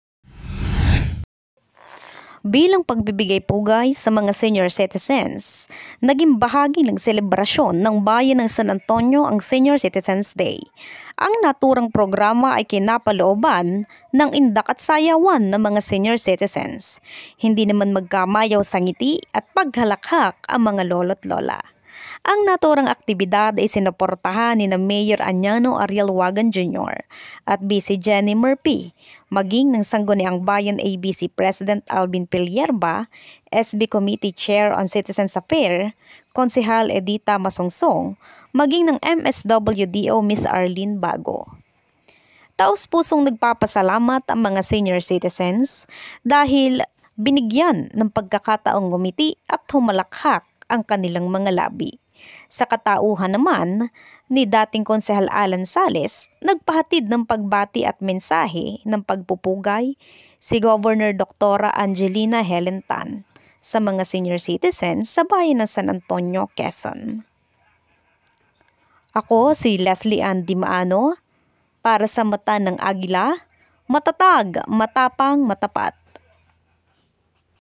Senior Citizens Day, ipinagdiwang sa bayan ng San Antonio, Quezon
Bilang pagbibigay-pugay sa mga senior citizens, naging bahagi ng selebrasyon ng bayan ng San Antonio ang "Senior Citizen Day".

Ang naturang programa ay kinapalooban ng indak at sayawan ng mga senior citizens. Hindi naman magkamayaw ang ngiti at paghalakhak ng mga lolo at lola.

Ang naturang aktibidad ay sinuportahan nina Mayor Aniano Ariel Wagan at Bise-Mayor Jenny Murphy maging ng sangguniang bayan ABC President Alvin Pillerba, SB Committee Chair on Citizens Affair Kgg. Editha Masongsong at ng MSWDO Ms. Arlene Bago.

Taos-pusong nagpapasasalamat ang mga senior citizens sa nasabing aktibidad na nagdulot sa kanila ng labis na kasiyahan.